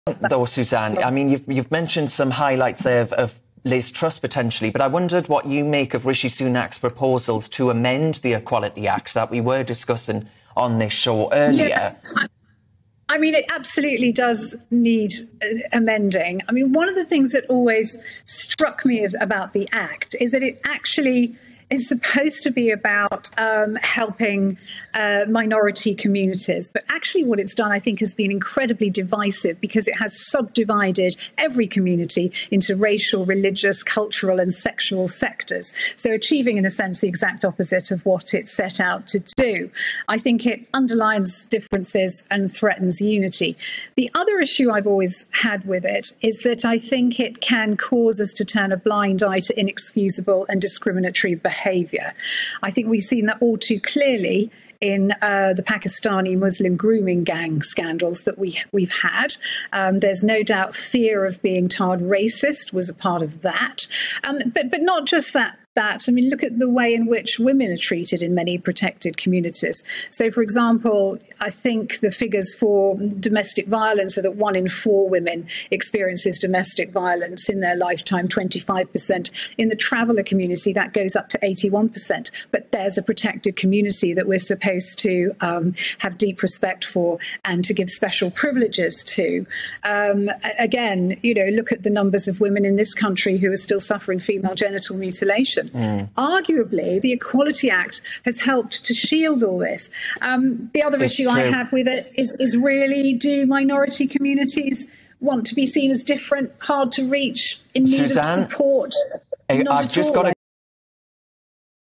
Is it time to ditch the Equality Act? | Suzanne Evans discusses
‘It has achieved the exact opposite of what it set out to do.’

Suzanne Evans takes aim at the Equality Act following Rishi Sunak’s pledge to review the legislation.